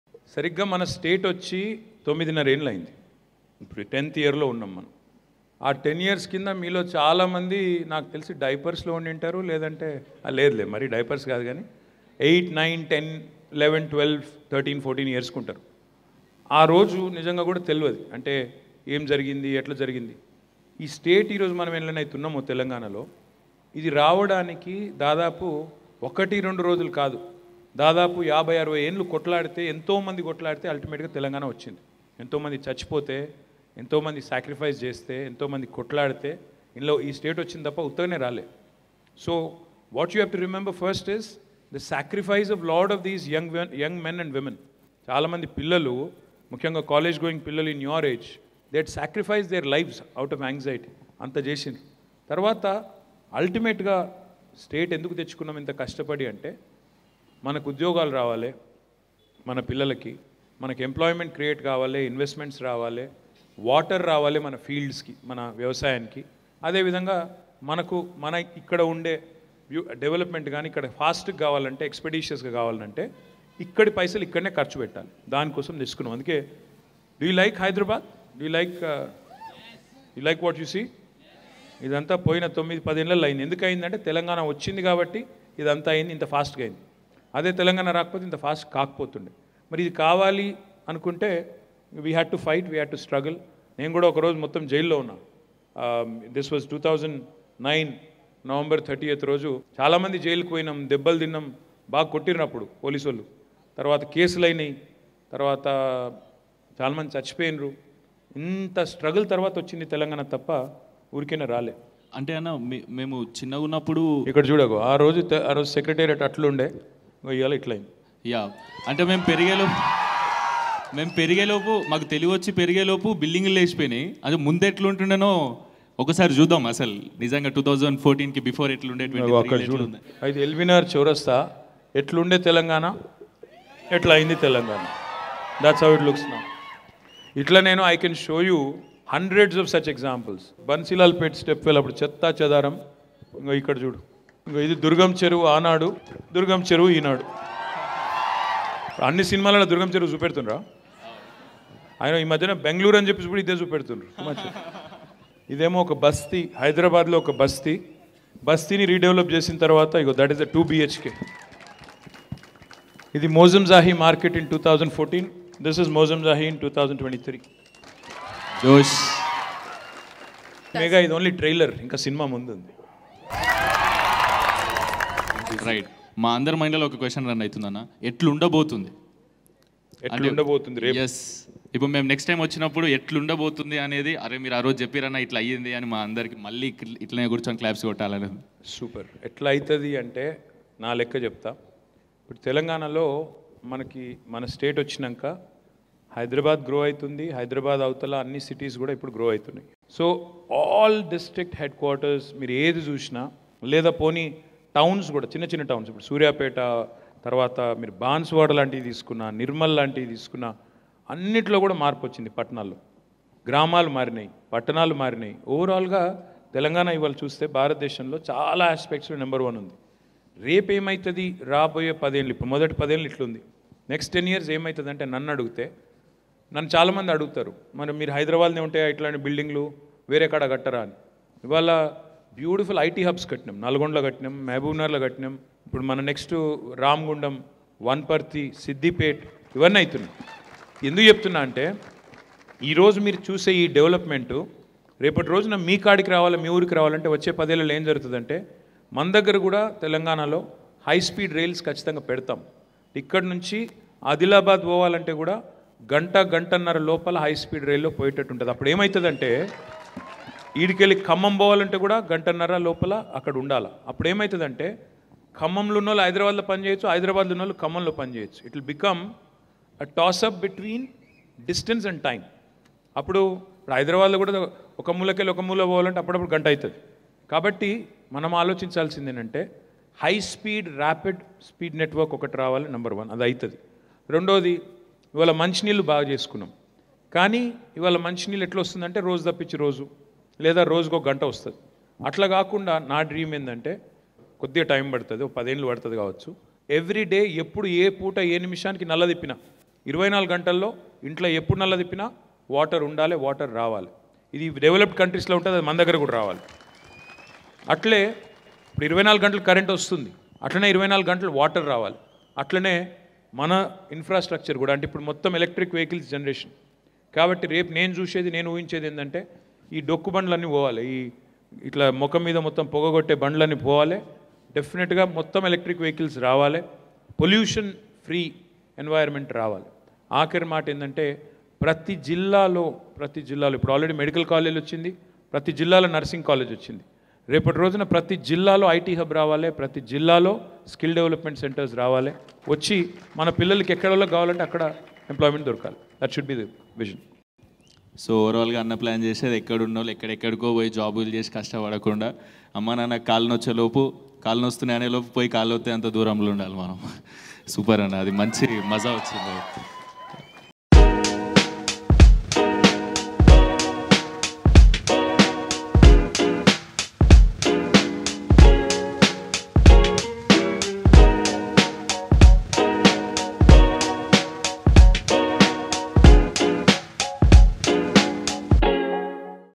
2014 vs 2023 BRS హైదరాబాద్ ని ఎంత మార్చిందో చుడండి | Minister KTR interacted with First Time Voters
2014 vs 2023 BRS హైదరాబాద్ ని ఎంత మార్చిందో చుడండి | Minister KTR interacted with First Time Voters | NewsGlitz